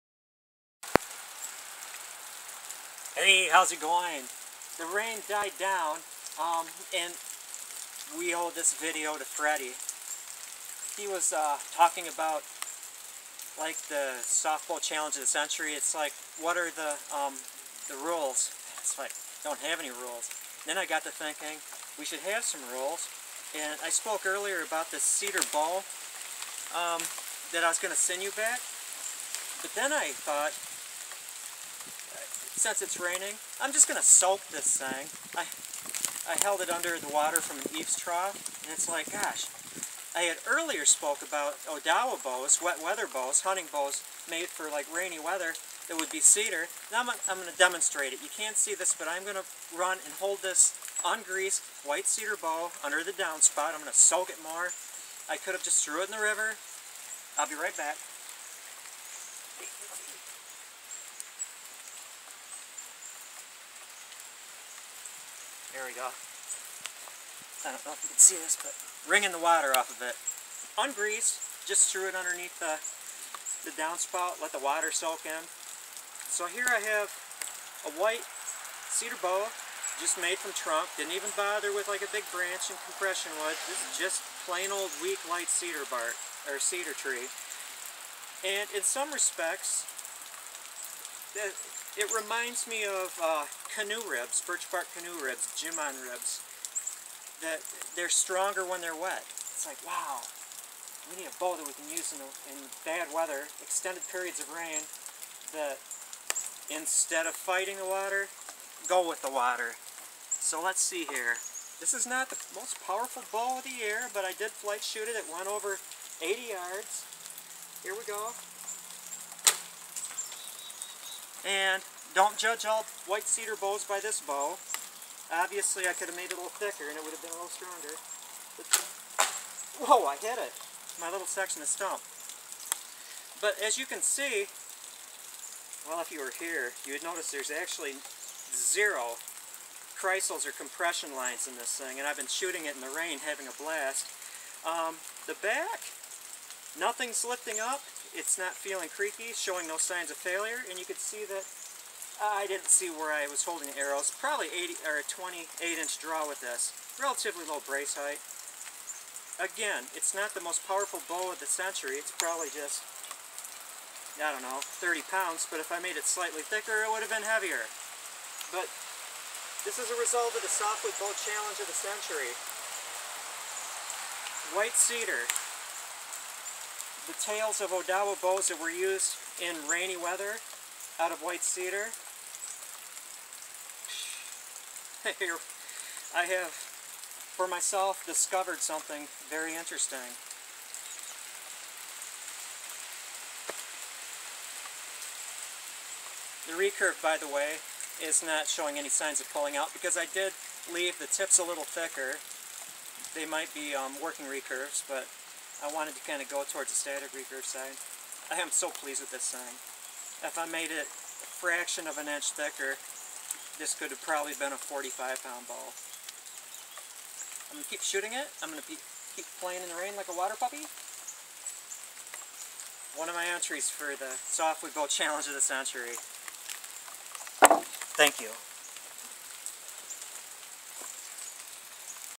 White Cedar Recurve Self Bow: Wet Weather Bow!
This is a wet weather bow made from White Cedar.I made it way too light though I may shorten it to raise the draw weight. It's crowning attribute is that it is a wet weather bow. In fact, soaking it in water before using makes it more limber. I should call it my Jiiman Rib Bow (Canoe Rib Bow). It is 66" tip to tip and after 50 arrows shot through it, has gained no measurable string follow.